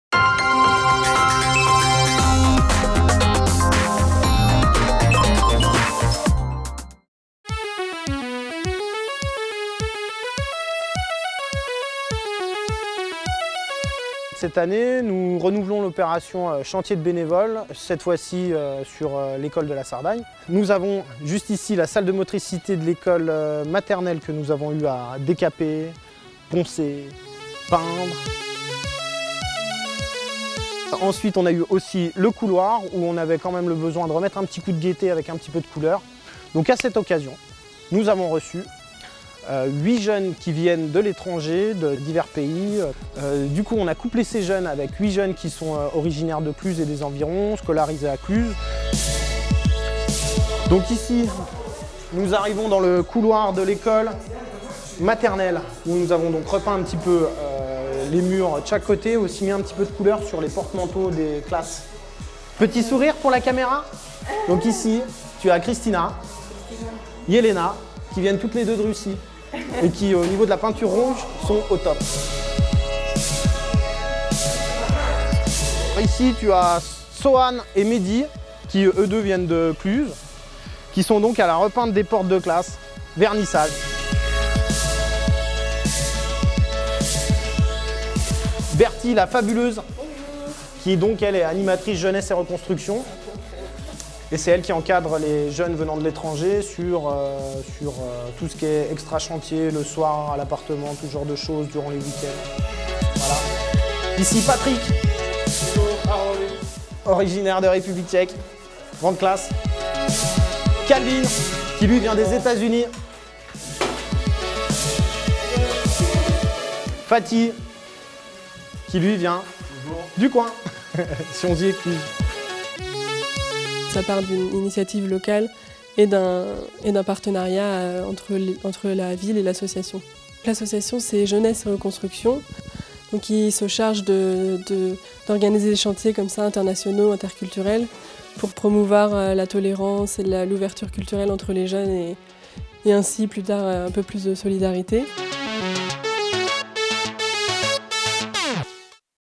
Chantiers Internationaux
Pendant le mois de juillet, 8 jeunes clusiens et 8 jeunes bénévoles de l'association jeunesse et reconstruction participent à la rénovation de l'école primaire de la Sardagne.
Sensibilisés au projet les jeunes de Cluses ont à cœur de faire
découvrir les alentours aux jeunes bénévoles étrangers.
Un reportage réalisé par Canal C TV, Web TV et télévision locale du bassin clusien, Haute-Savoie.